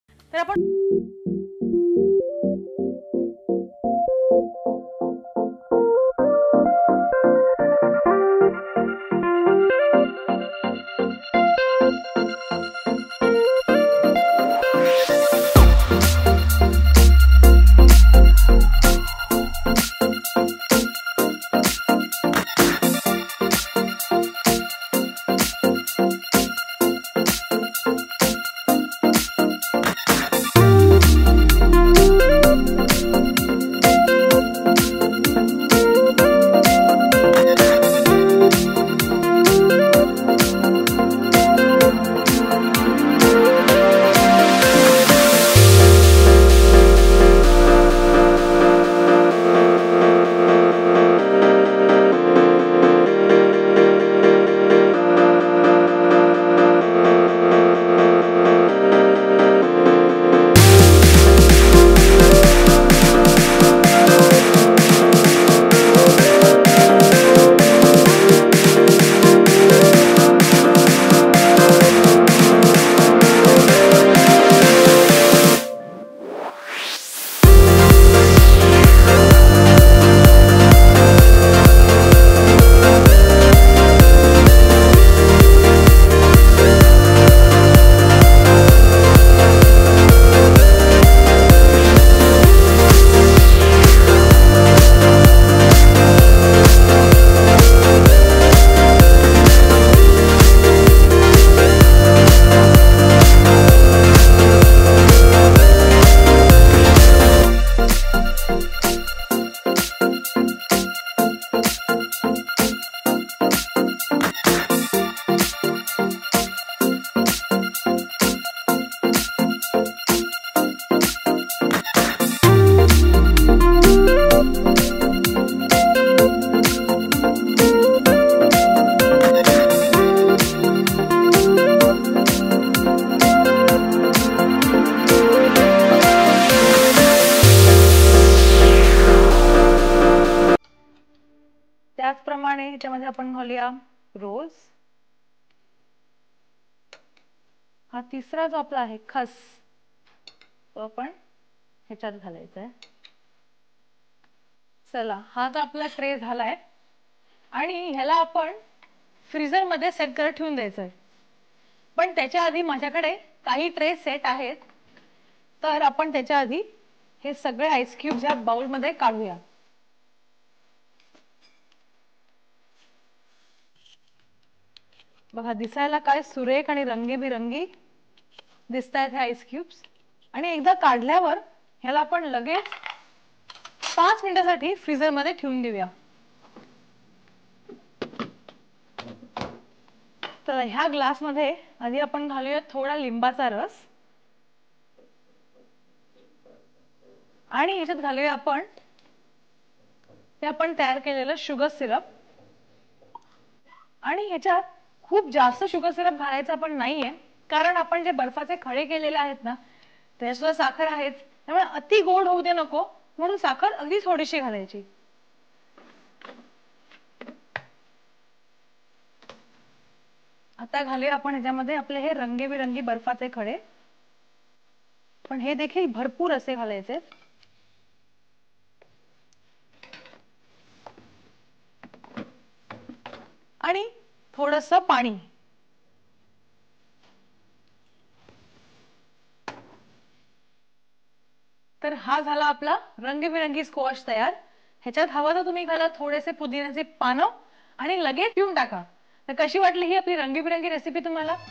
Colourful Squash
Refreshing summer drink recipe in Marathi with English subtitles